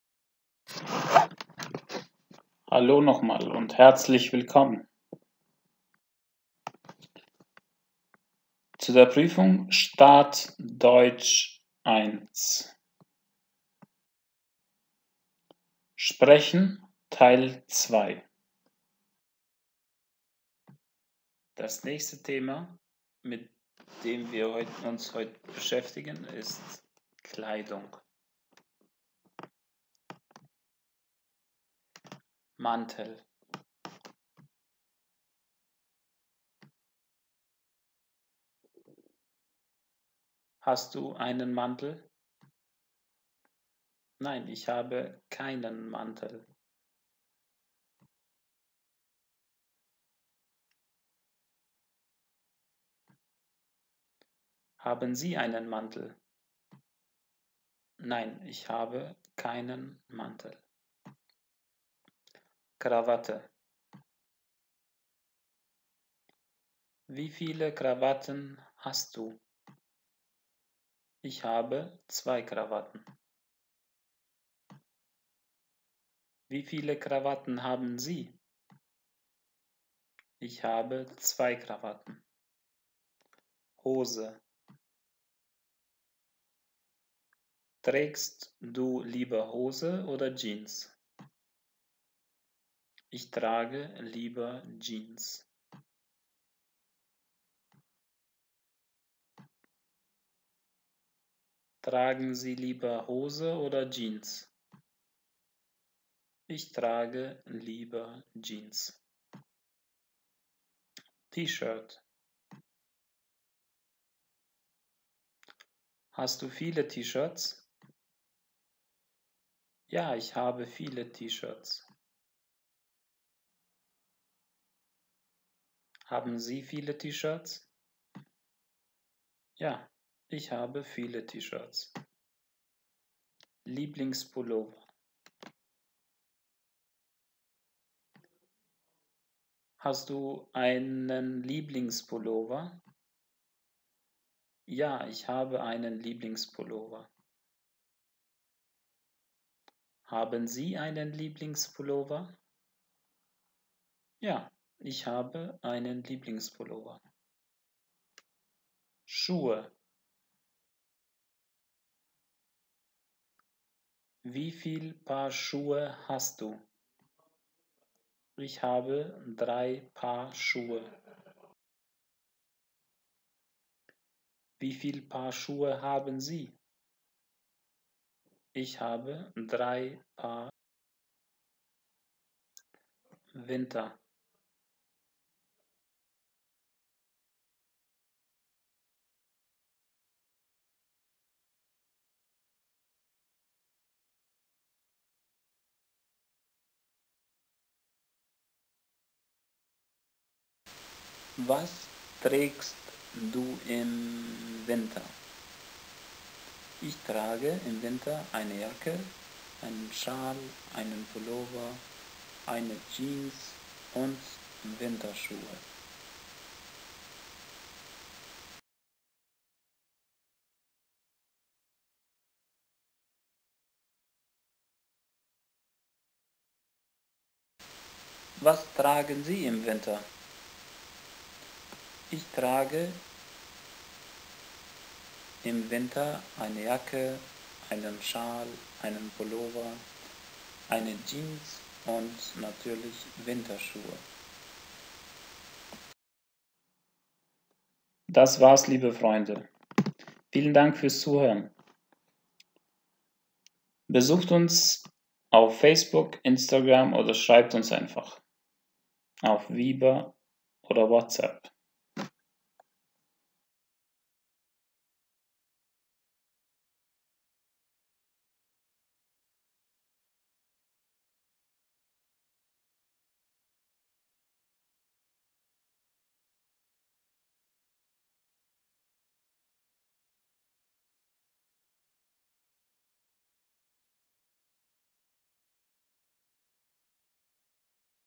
Start Deutsch 1, Sprechen Teil 2 Thema: Kleidung
Sprechen Teil 2 Thema: Kleidung

Mantel:
•Hast du einen Mantel?
-Nein, ich habe keinen Mantel.
•Haben Sie einen Mantel?

Krawatten:
•Wie viele Krawatten hast du?
-Ich habe zwei Krawatten.
•Wie viele Krawatten haben Sie?

Hose:
•Trägst du lieber Hose oder Jeans?
-Ich trage lieber Jeans.
•Tragen Sie lieber Hose oder Jeans?

T-Shirts:
•Hast du viele T-Shirts?
-Ja, ich habe viele T-Shirts.
•Haben Sie viele T-Shirts?

Lieblingspullover:
•Hast du einen Lieblingspullover?
-Ja, ich habe einen Lieblings-pullover.
•Haben Sie einen Lieblings-pullover?

Schuhe:
•Wie viel Paar Schuhe hast du?
-Ich habe drei Paar Schuhe.
•Wie viel Paar Schuhe haben Sie?

Winter:
•Was trägst du im Winter?
-Ich trage im Winter eine Jacke, einen Schal, einen Pullover,       eine Jeans und Winterschuhe

•Was tragen Sie im Winter?